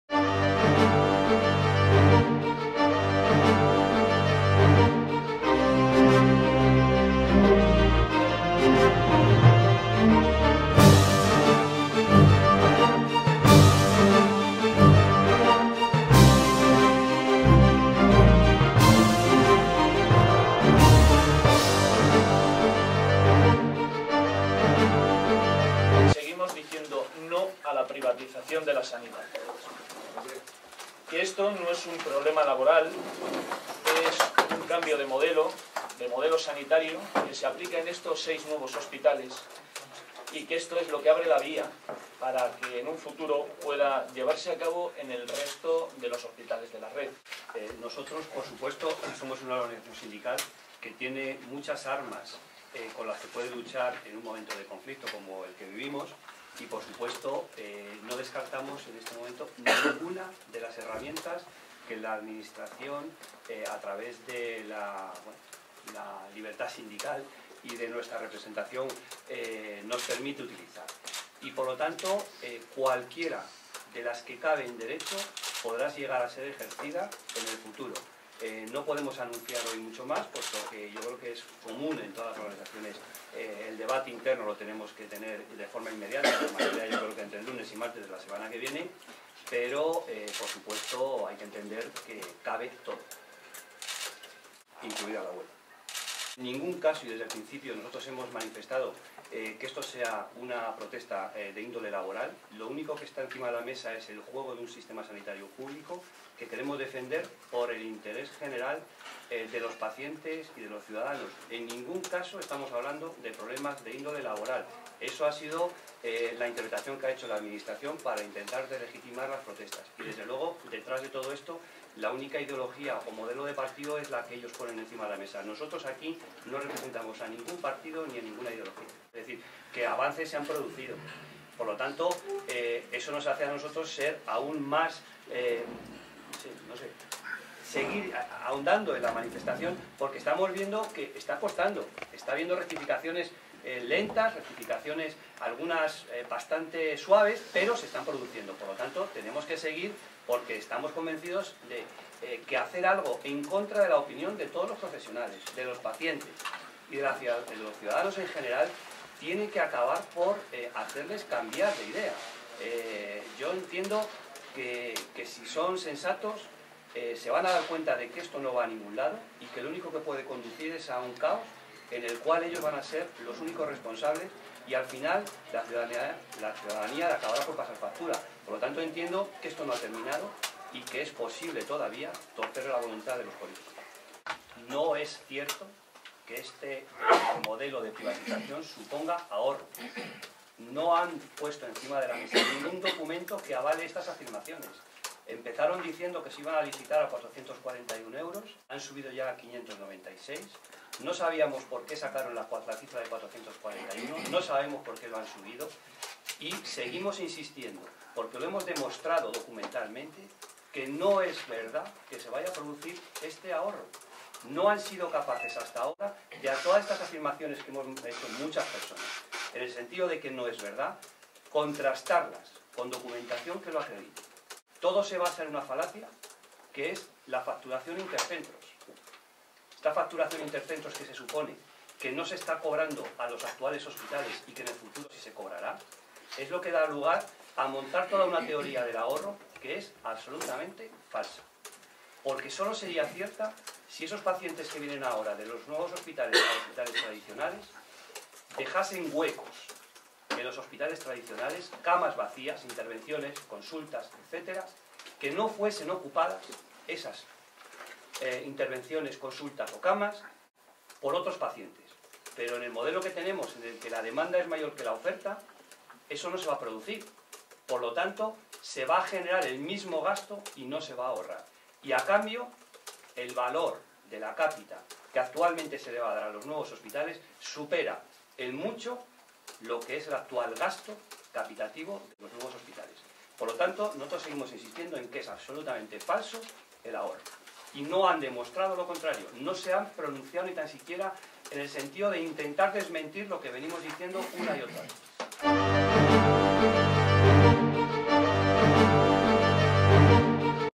E-REVISTA de AMYTS Nº 29. Rueda de prensa OOSS Mesa Sectorial del 19-04-2013
El 19 de abril de 2013 las organizaciones sindicales de la Mesa Sectorial de Sanidad de la Comunidad de Madrid (AMYTS, SATSE, USAE, CCOO, UGT y CSIT-UP) en una rueda de prensa conjunta rechazaron los pliegos dados a conocer por la Comunidad de Madrid para privatizar las gestión seis hospitales. Dichos pliegos se van a recurrir judicialmente, no se descarta ninguna medida de presión, incluida la huelga, y el secretario general de AMYTS, Julián Ezquerra hizo especial hincapíe en demostrar que los datos ofrecidos por la Consejería se sustentan en una "falacia" y son "falsos".